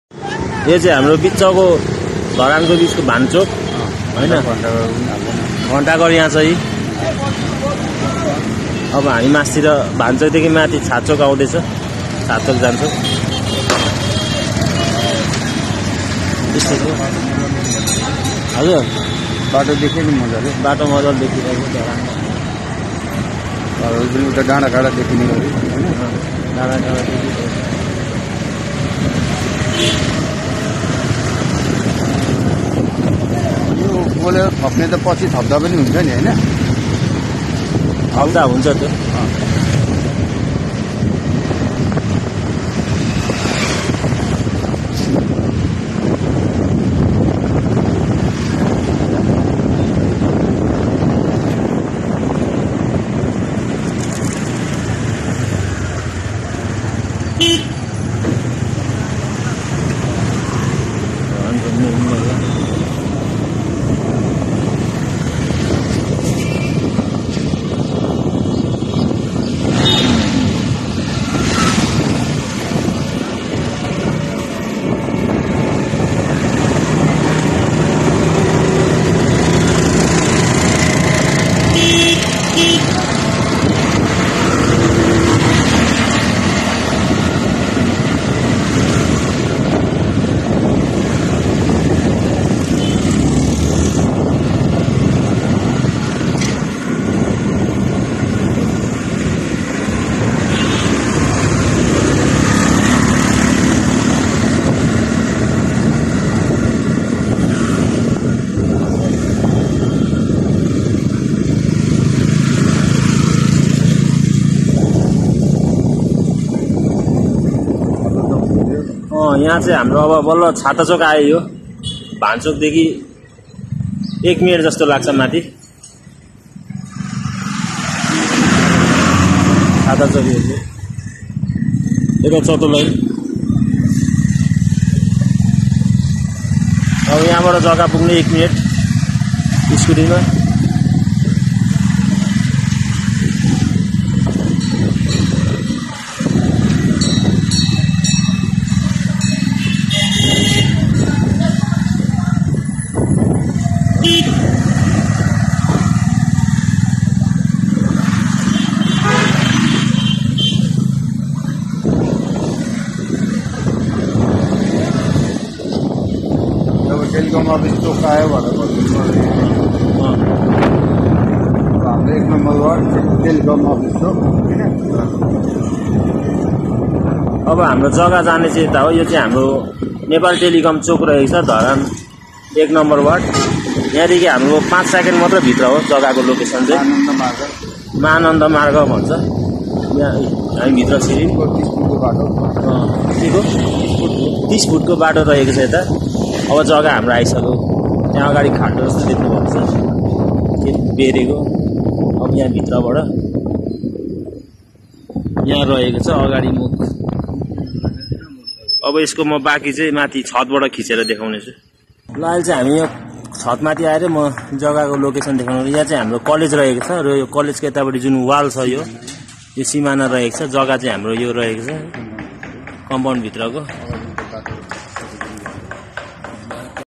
Land at Dharan-1 Near Nepal Telecom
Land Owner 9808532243

This Beautiful land of Dharan Nepal Telecom. is located in a prime and peaceful location. This land is facing towards the North direction with having a land area of 1 Kattha 7 Dhur.

Facilities:

100m from the main road,

Access road: 30ft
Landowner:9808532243